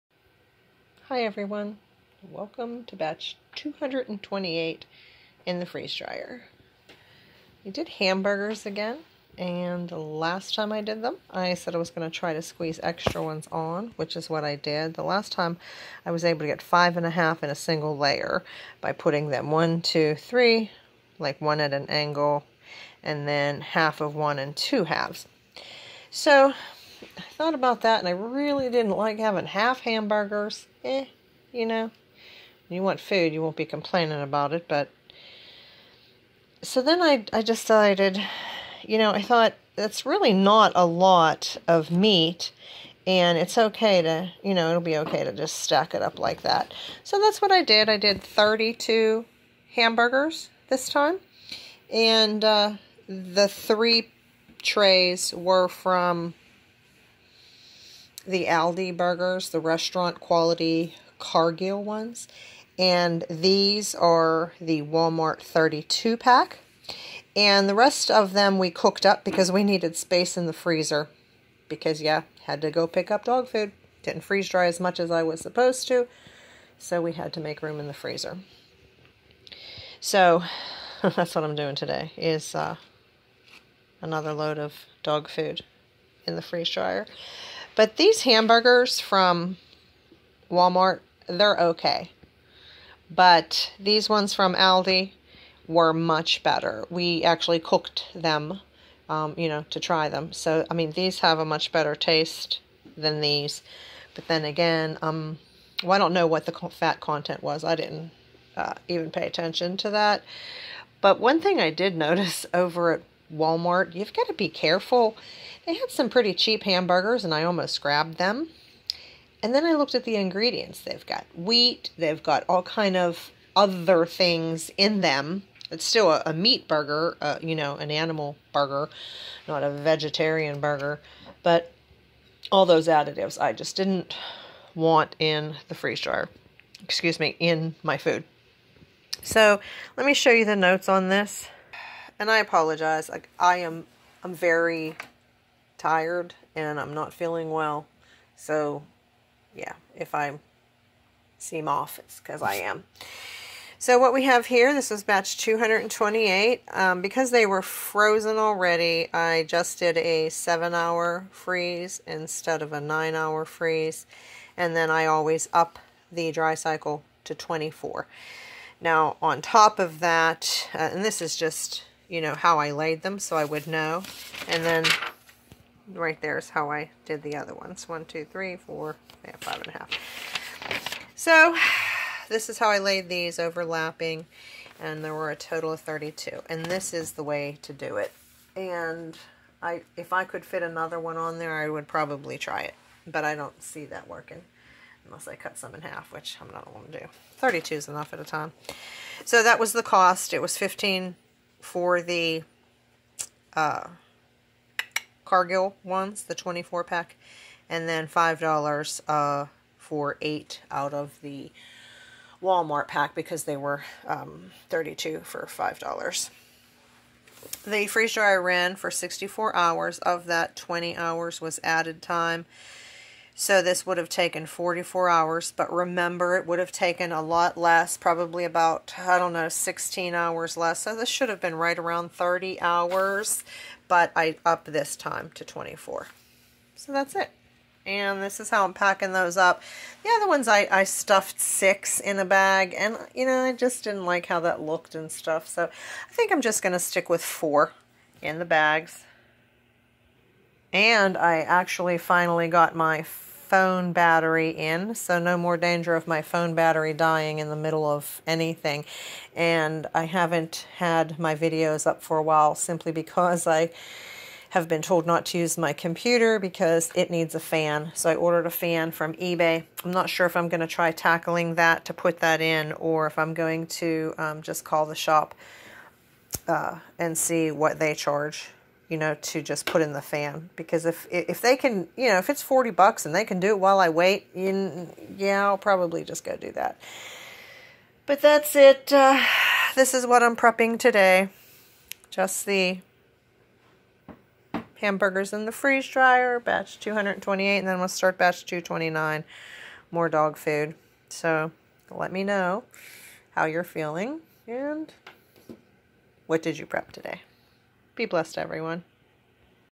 MORE Raw burgers in the Harvest Right Freeze Dryer, Batch 228, What did YOU prep today?
More burgers in the Harvest Right Freeze Dryer.  I didn't cut these ones, and I layered them.  Come see how it worked! See you in SHTF, my Preparedness Family and Community,

Please like, follow, and share if you can!
 20200428_B228MoreRawBurgers.mp4